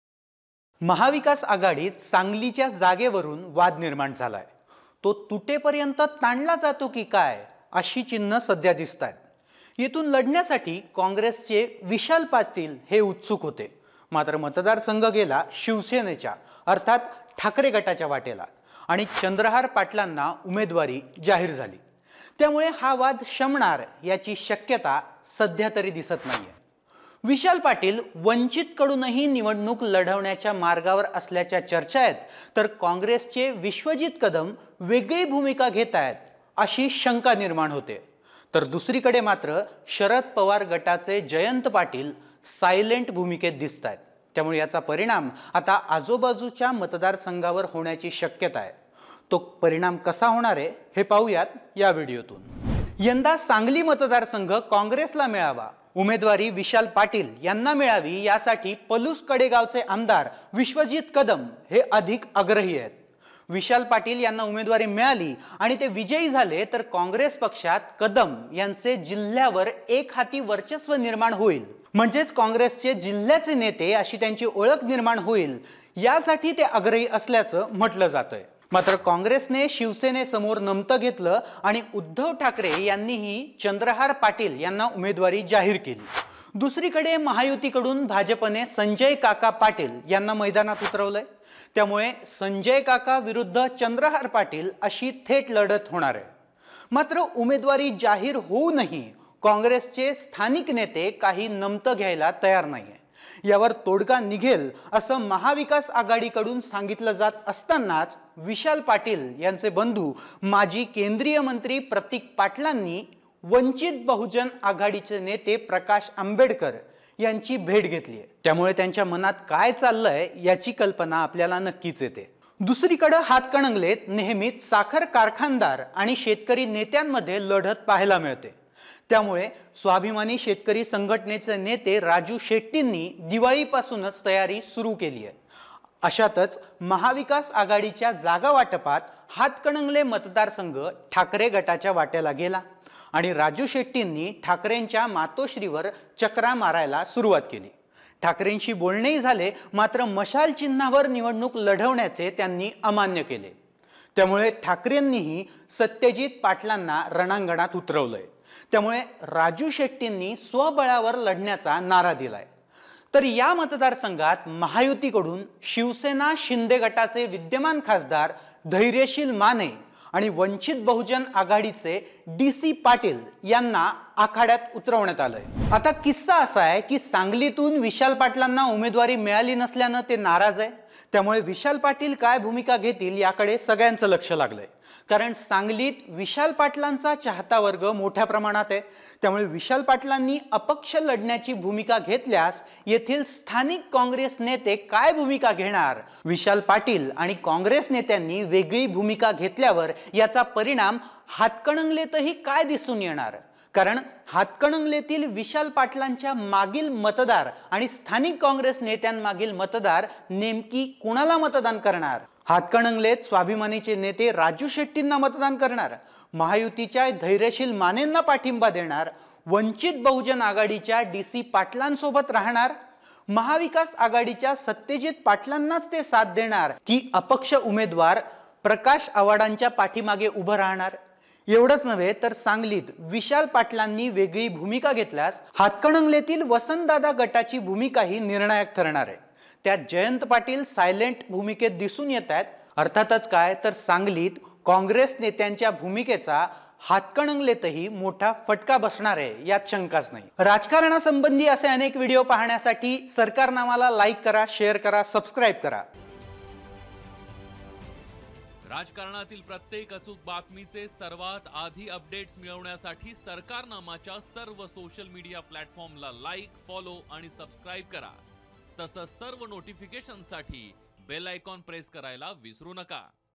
विशाल पाटील सांगलीतच नाहीतर हातकणंगलेतही करणार ठाकरेंचा गेम ? | Vishal Patil
(KB0324)
(OG0324)